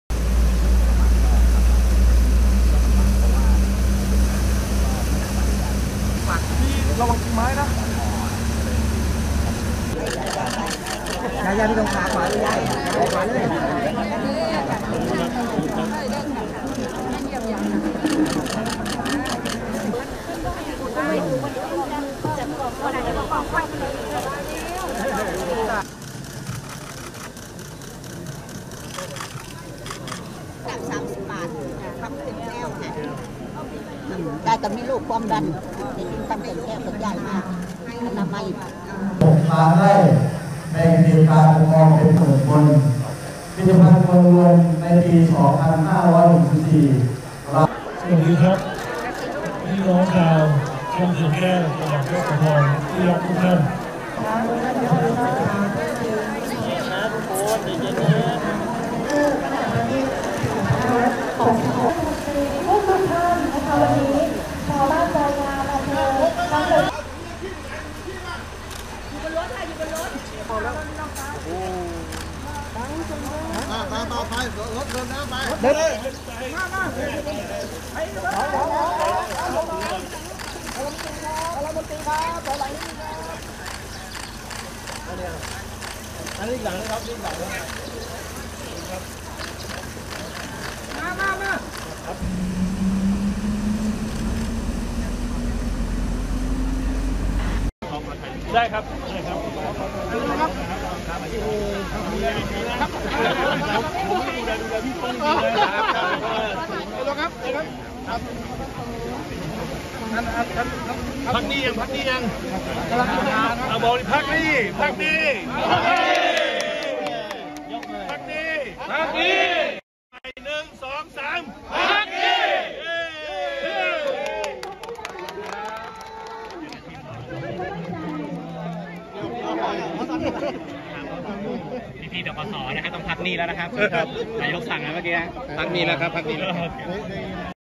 ‘เศรษฐา‘ นำ จนท.ธกส.ชูกำปั้น ’พักหนี้‘ ระหว่างลงพื้นที่ยโสธร
ผู้สื่อข่าวรายงานว่าช่วงหนึ่งระหว่างที่นายเศรษฐา ทวีสิน นายกรัฐมรตรี และ รมว.คลัง ลงพื้นที่พยปะปะประชาขนแบะมอบสิ่งของช่วยเหลือ ที่บ้านทรายงาม ตำบลกุดกุง อำเภอคำเขื่อนแก้ว จังหวัดยโสธร ได้แวะทักททยกับหน่วยแพทย์เคลื่อนที่ที่มาออกหน่วยบริการประขาชน โดยหลังจากรับฟังรายงานเสร็จนายกรัฐมนตรีหันกลับไปถามแพทย์ว่ามียาติดมาหรือเปล่า ให้เอายามาแจกจ่ายชาวบ้านเลย “พาผมพาไปดูเลยได้หรือไม่ พาไปดูเลยมา” จากนั้นนายกฯจูงมือพยาบาลไปยังที่ๆชาวบ้านนั่งอยู่ เพื่อไปมอบยาสามัญประจำบ้านให้กับชาวบ้าน โดยนายกฯ ย้ำกับหน่วยแพทย์ว่าดูแลชาวบ้านหน่อย